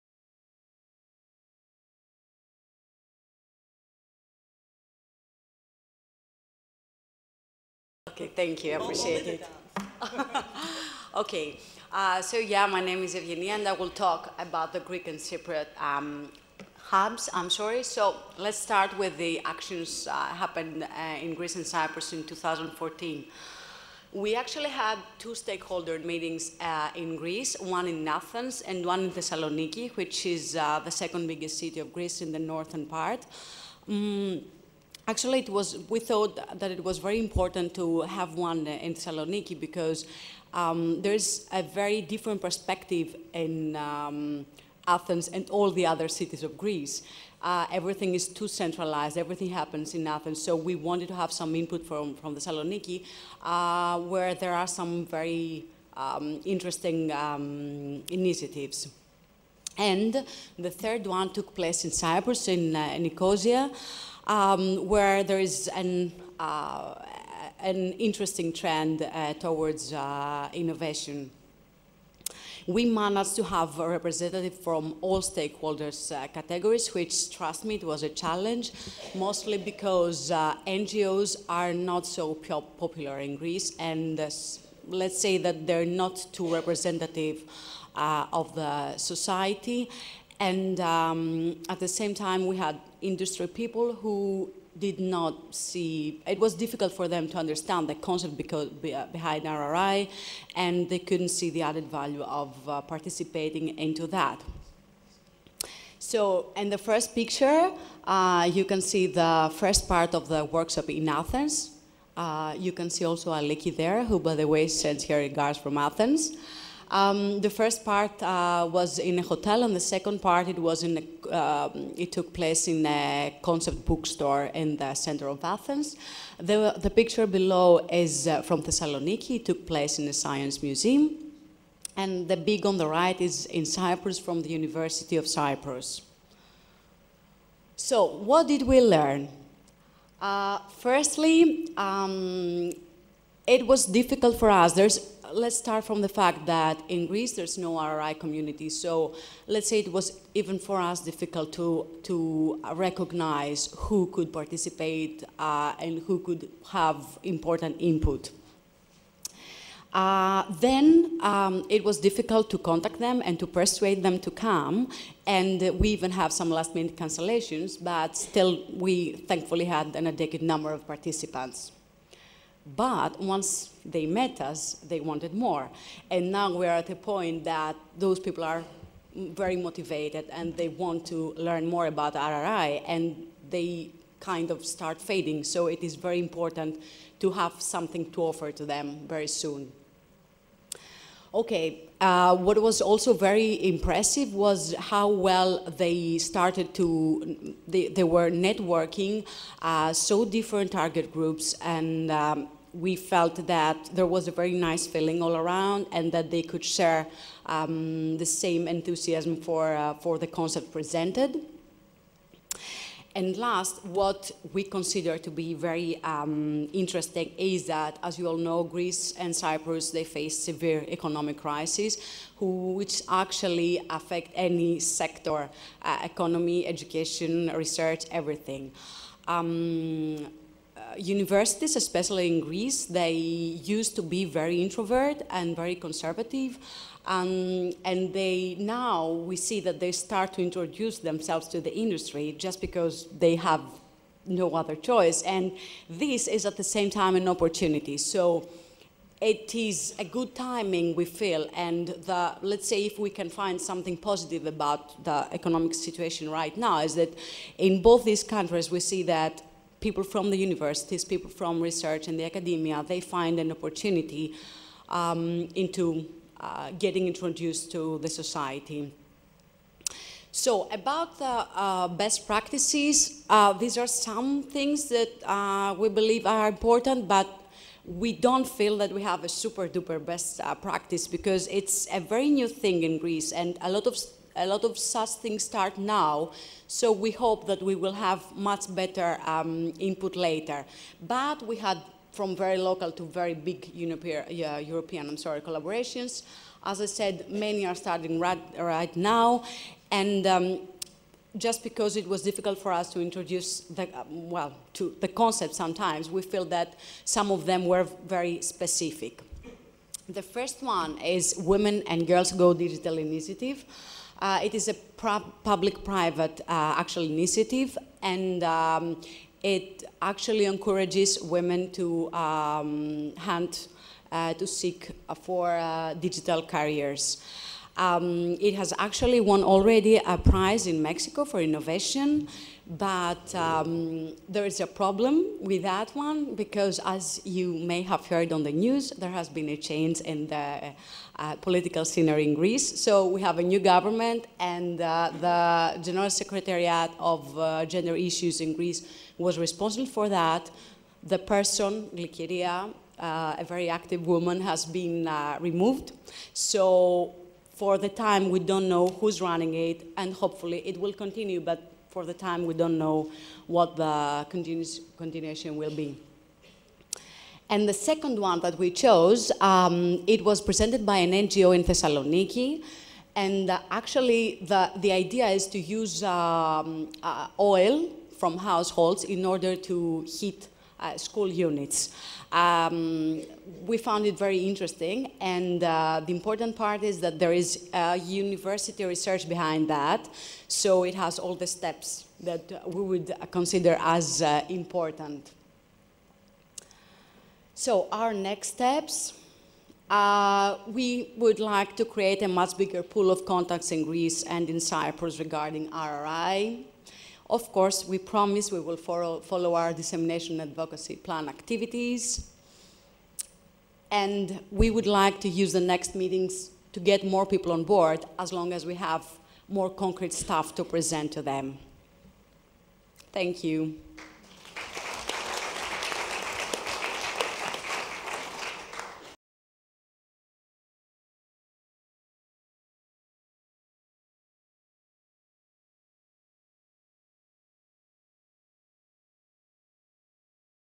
RRI in Greek and Cypriot contexts
During autumn 2014, Ellinogermaniki Agogi organized stakeholder workshops in both Cyprus and Greece. In order to get a representative sample of participants two workshops were held in north of Greece and Athens. Participants represented all stakeholder groups, and promising practices presented by all attendees. As both countries are in the middle of an economic crisis, promising practices seemed to focus on issues that would look into the innovation aspect much more than in the past, and would look at aspects not well embedded in the culture of Greek and Cypriot society such as involvement in civil society associations, small scale initiatives involving different partners, new collaborations etc. Promising practices in our context might not well represent all aspects of RRI as it is regularly done in other European countries but there is certainly a tendency to experiment with new initiatives and to connect with previously  very much disconnected stakeholders (see for example universities and their connection with industries, CSOs, policy makers etc.).

The European project RRI Tools aims at building a Responsible Research & Innovation Toolkit, with the concerned stakeholders, to make RRI happen in real life!